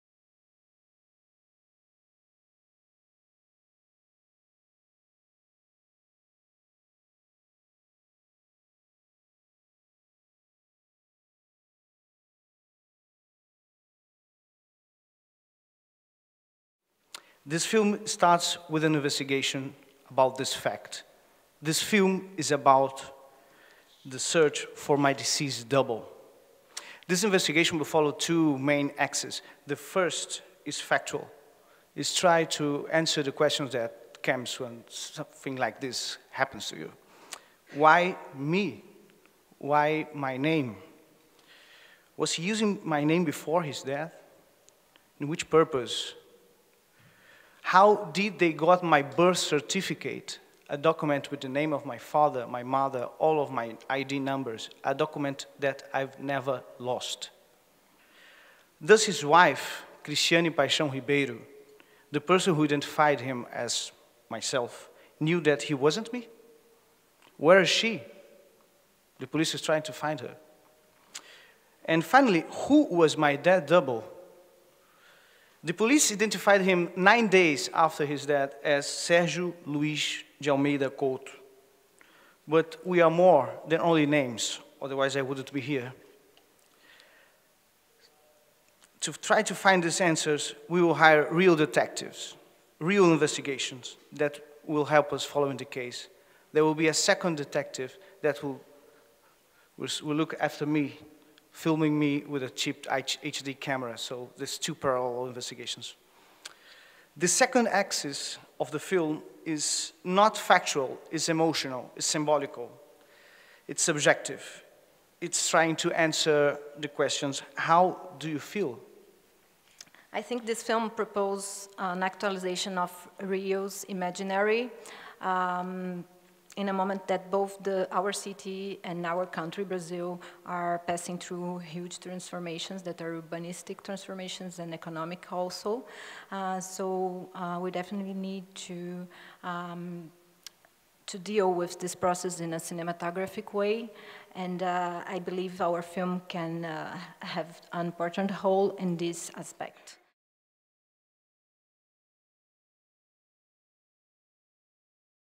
Biennale College Cinema 2013 - The Death of J.P. Cuenca
João Paulo Cuenca (director) and Marina Meliande (producer) comment on their project "The Death of J.P. Cuenca" that was selected for the first phase of the Biennale College - Cinema